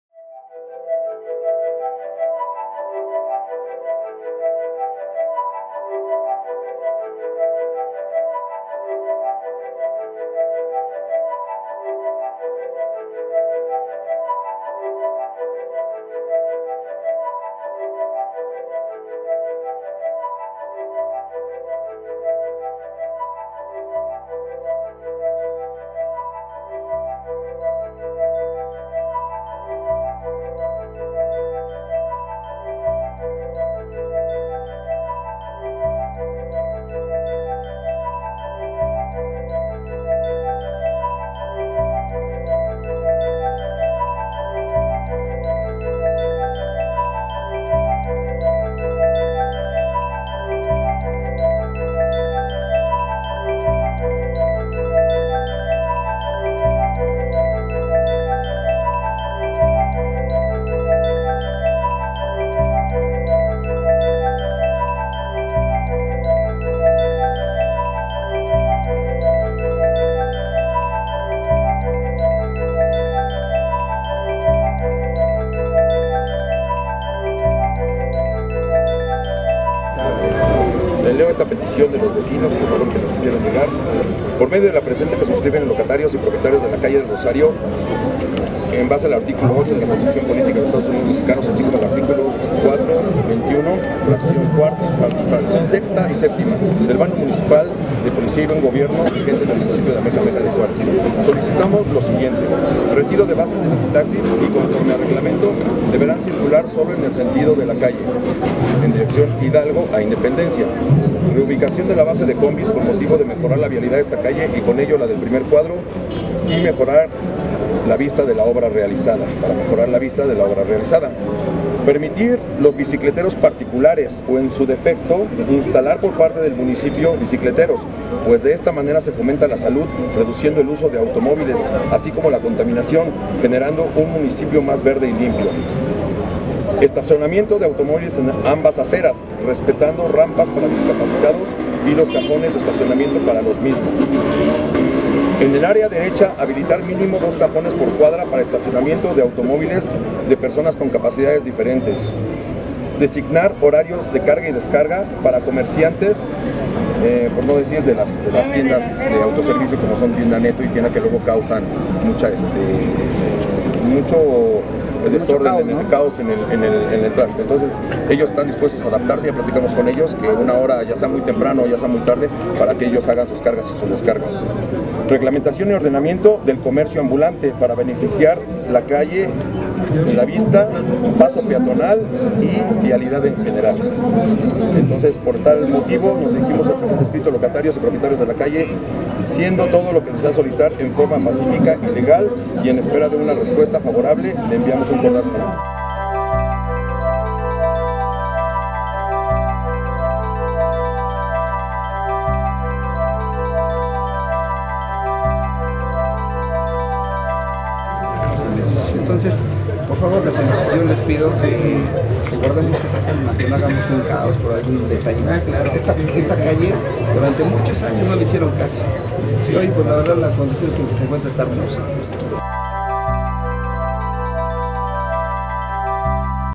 A destiempo y con deficiencias en la obra, reabren la calle Rosario
La tarde del 15 de enero, el presidente municipal de Amecameca, Miguel Ángel Salomón Cortés, dio apertura a la obra "Repavimentación de la calle Rosario", la cual debía realizarse del 14 de octubre al 31 de diciembre de 2019, no obstante, vecinos y locatarios expresaron su inconformidad.